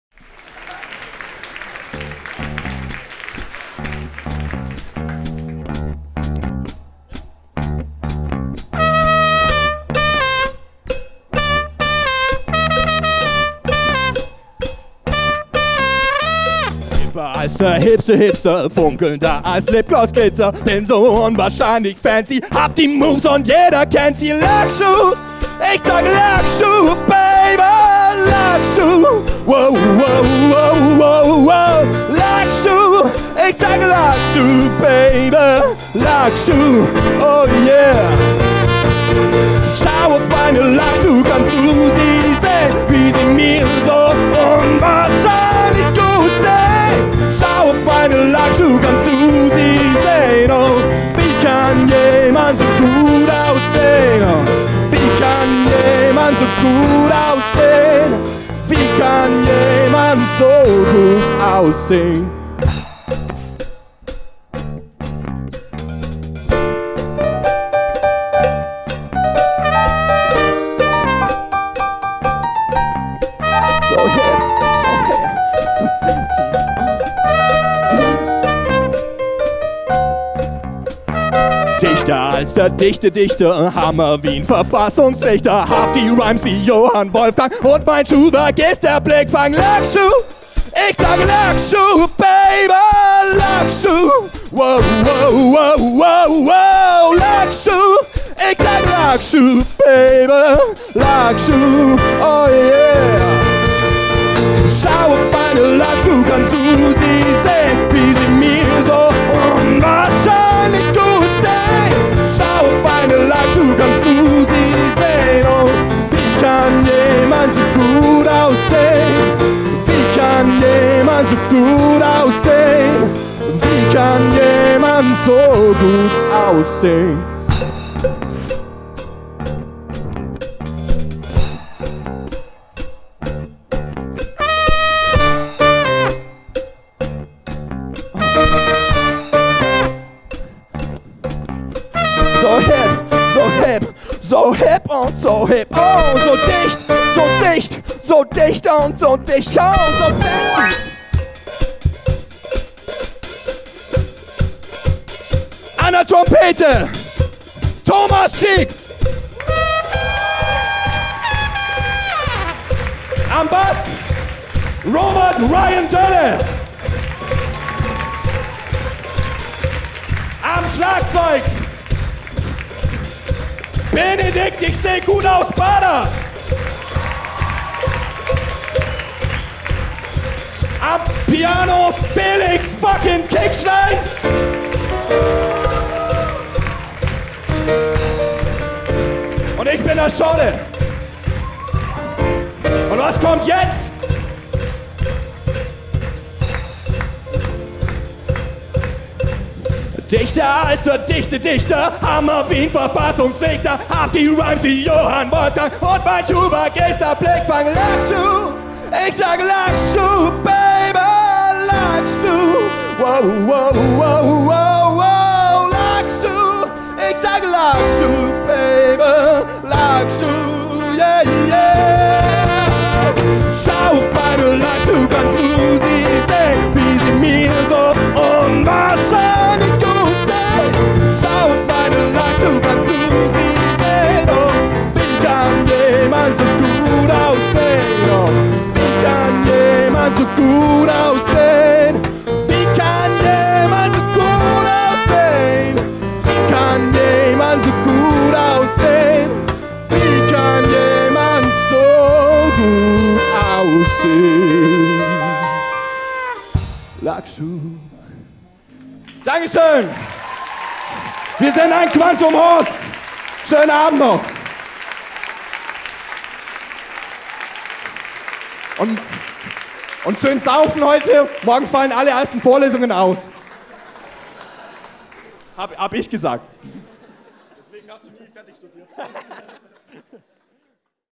Ein Quantum Horst bei CampusKunst am 07.12.2016
Ein Quantum Horst bei CampusKunst, der Kulturveranstaltung des Studentenwerks Augsburg für studierende Newcomer, am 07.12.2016
Mensa des Studentenwerks Augsburg an der Universität Augsburg